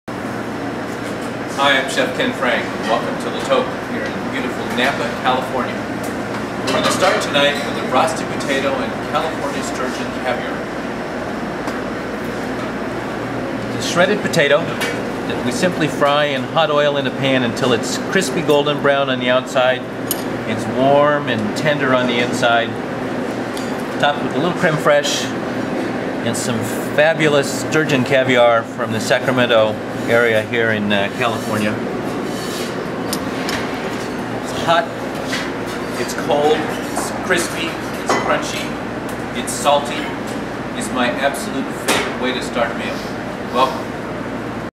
Ken Frank prepares a dish at La Toque
Chef Patron Ken Frank prepares a dish at his 1 Michelin star La Toque inside the Westin Verasa Napa hotel in downtown Napa, California.